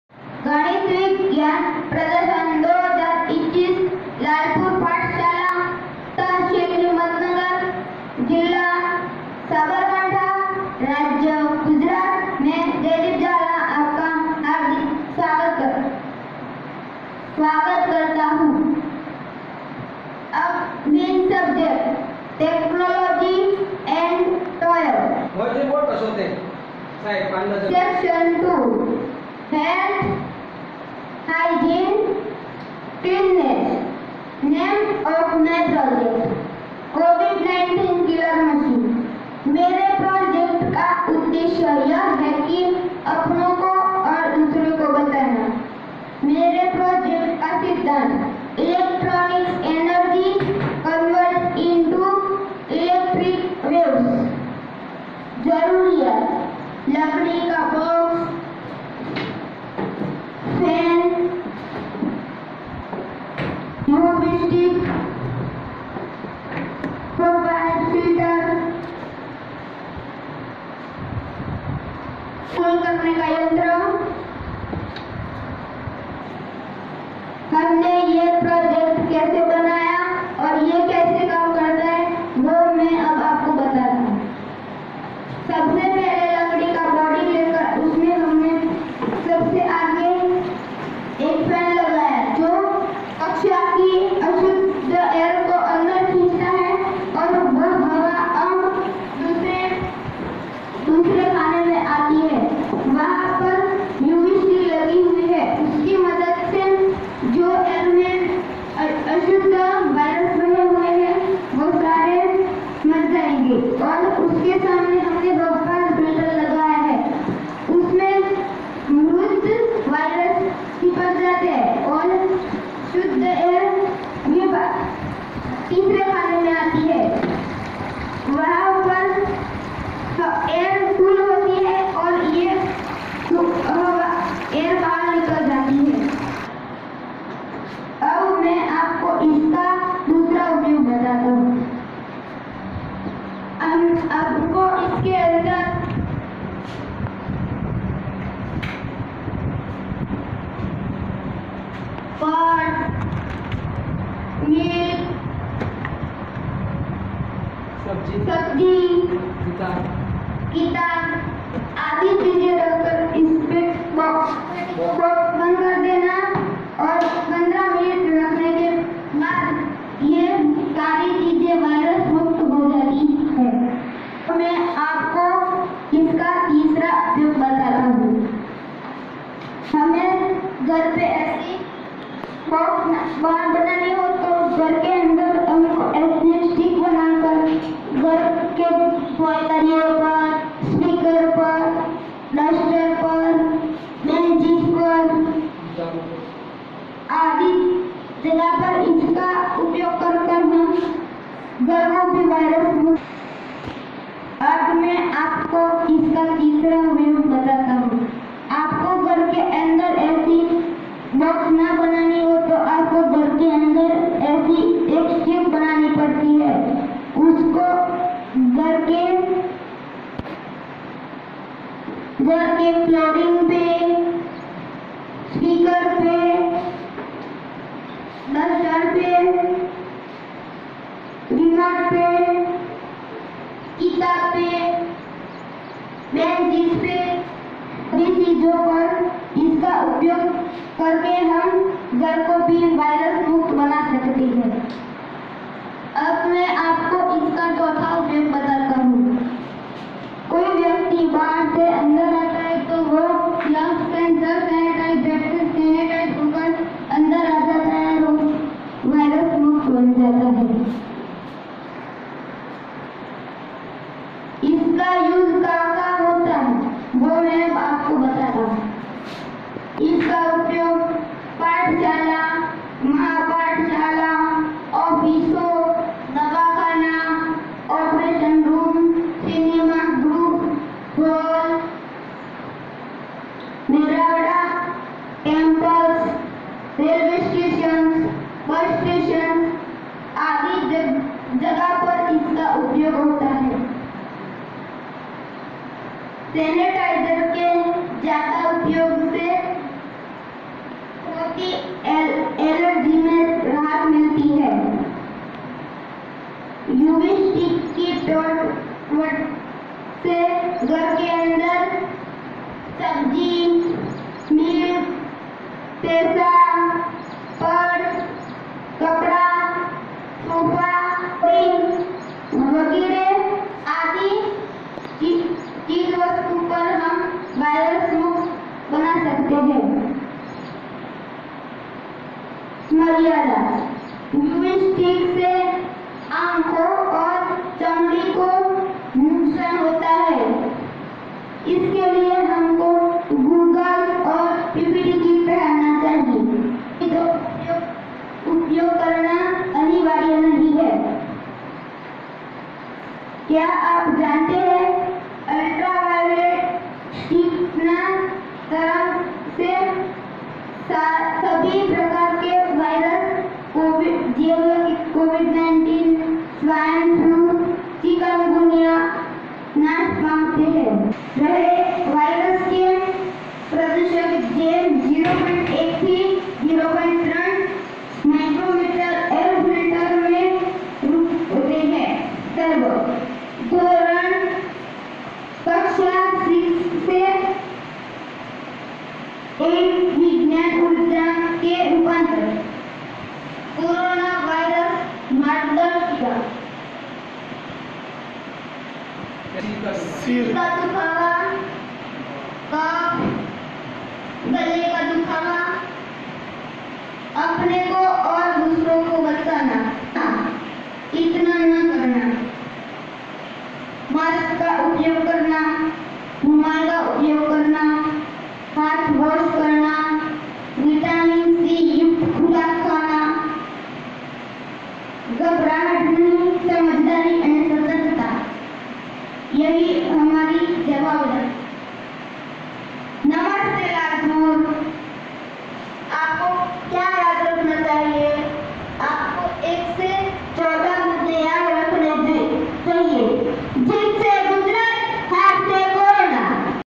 Covid 19 Killer Machine
This Model represents an air purifier it has two chambers; one chamber for trapping of microorganisms and in other chamber microorganisms are being killed by Ultraviolet rays.
यह मॉडल एक वायु शोधक को प्रदर्शित करता है, जिसका एक कक्ष सूक्ष्मजीवों को छानता है और दूसरे कक्ष में पराबैंगनी किरणों द्वारा सूक्ष्मजीवों को खत्‍म किया जाता है।